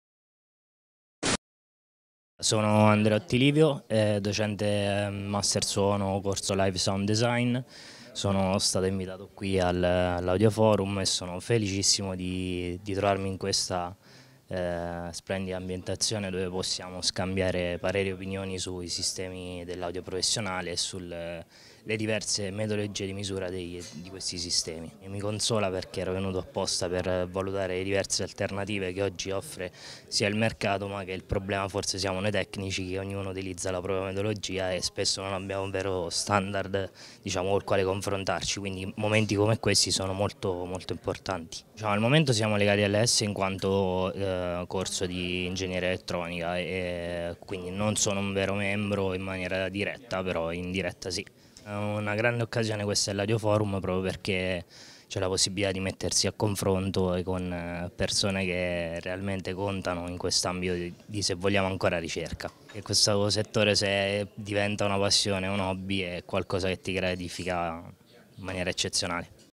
AudioForum @ ISE 2015 - Intervista a Livio Andreotti, docente Master Sound
Livio Andreotti sottolinea la possibilità di scambiare pareri e opinioni sull'audio professionale come elemento chiave della sua partecipazione all'AudioForum.